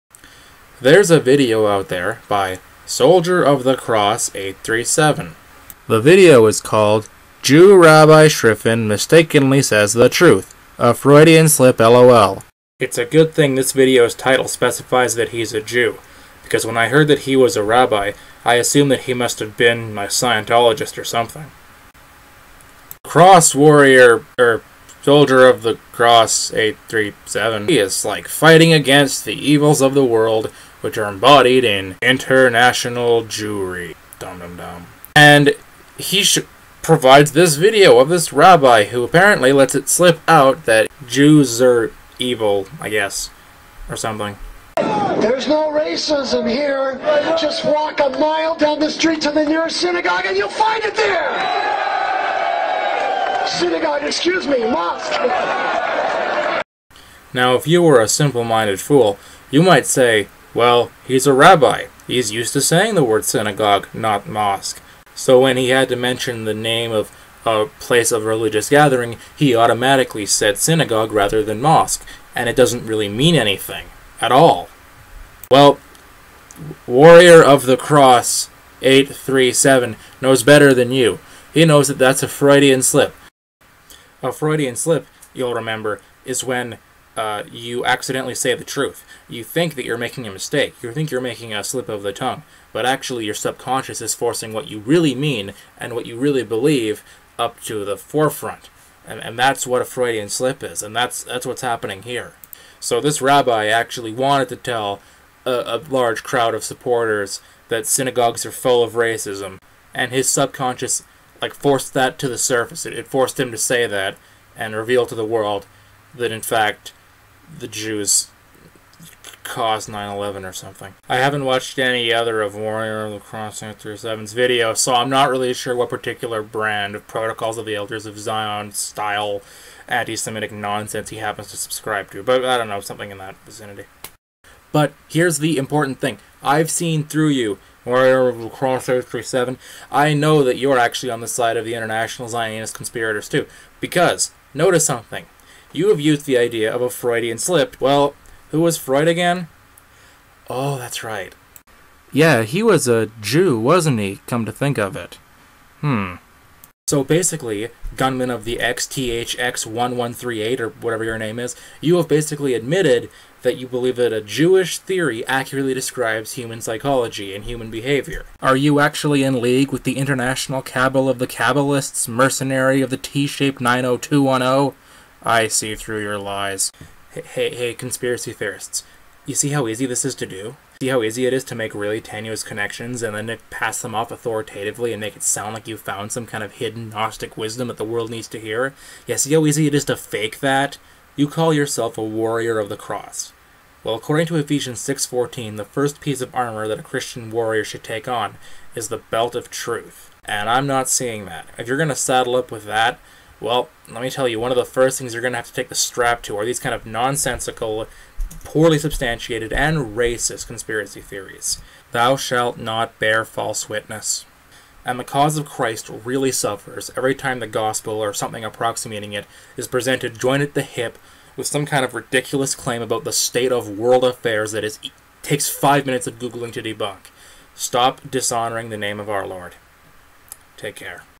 Conspiracy Theories: They Can Be Stupid.
This was a video I made really quickly, but some stuff I just can't let past without saying *something*.
You can consider this video a blanket response to everything that comes out of Brother Nathaniel's mouth.

And this is a lot more snark than refutation.  Because life is short.

Here's the original video, in case WOTC doesn't approve this as a video response: